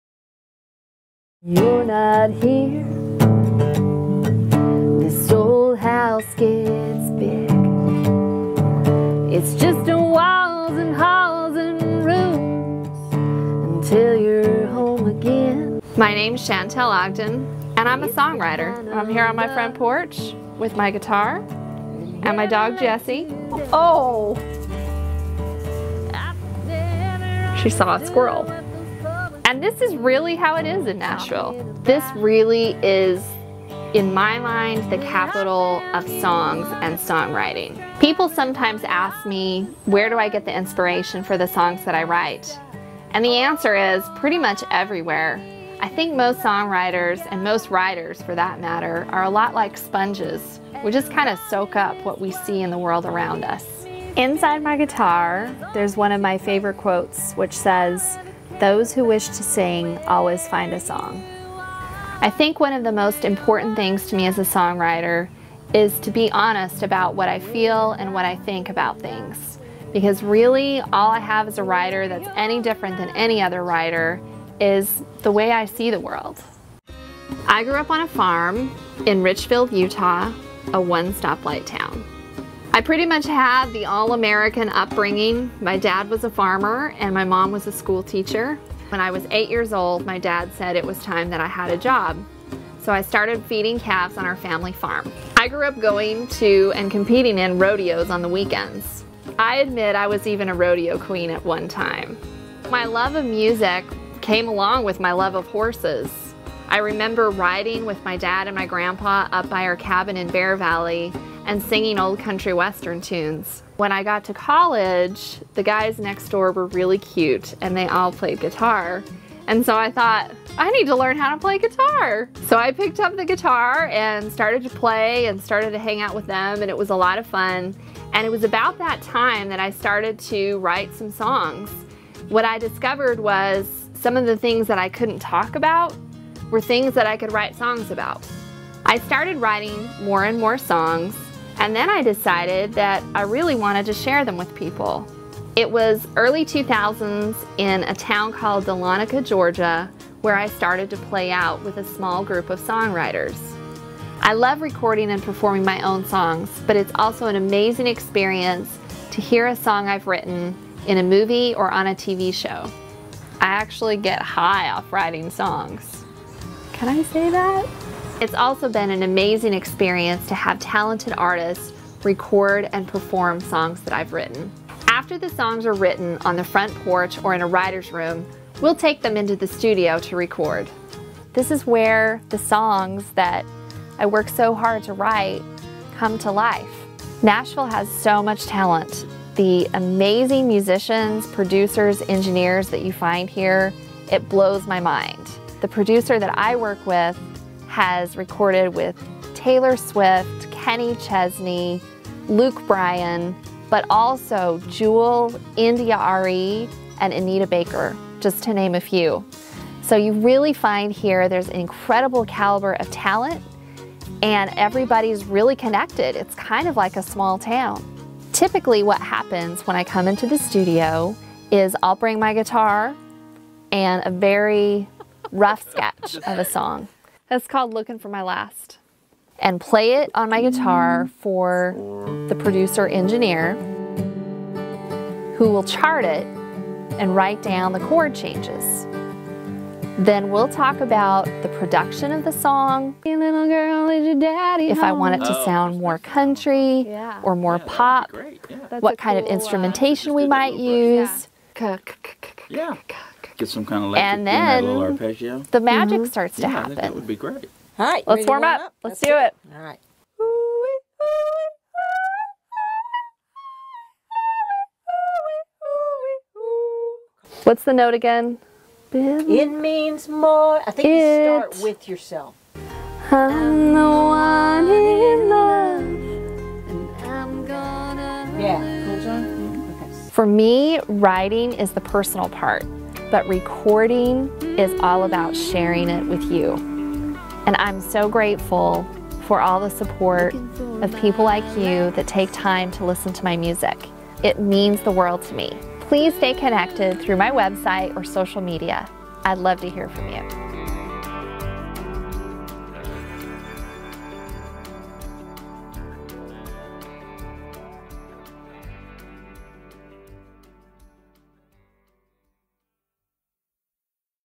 ShantellOgdenEPK
Get to know Nashville artist Shantell Ogden, from her front porch writing songs to the recording studio.